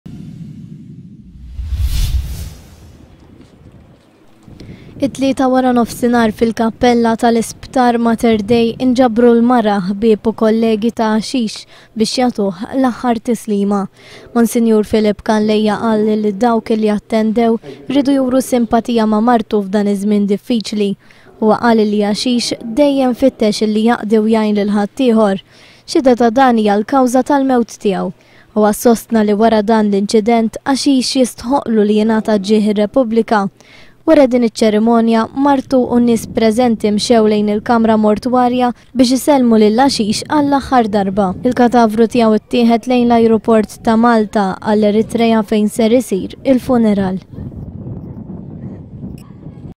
L-aħħar tislima lil eroj Ashih Tekleab Haile.
Illum inghatat l-ahhar tislima li Ashih qabel mal-katavru tieghu telaq lejn l-Eritreja.Dan Ir-ragel, deskritt bhala eroj, miet nhar l-erbgha biex  salva zewg studenti francizi.